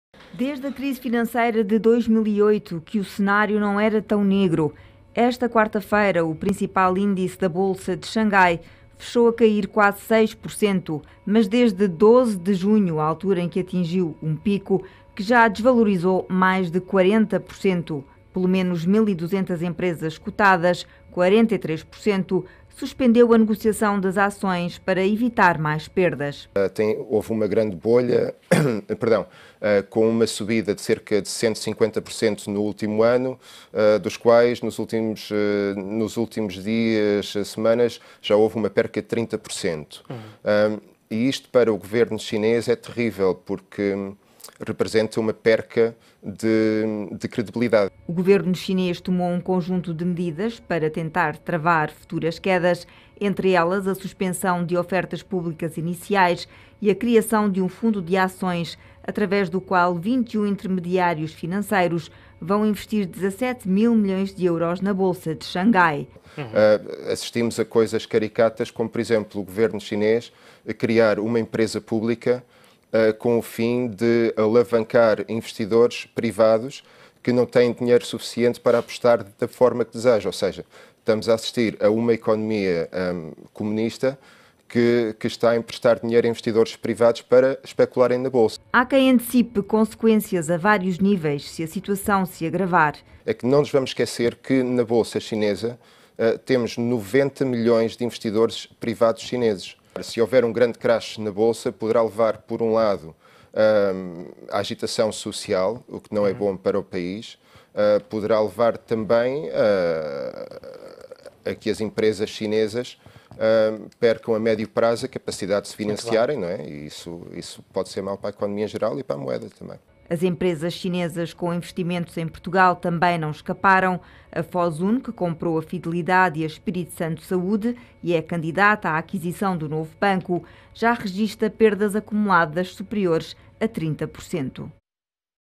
Pânico na bolsa chinesa, perigo para a economia mundial
A China serviu como uma muralha para a economia mundial durante as últimas crises, mas a queda violenta das acções é classificada como uma ameaça global.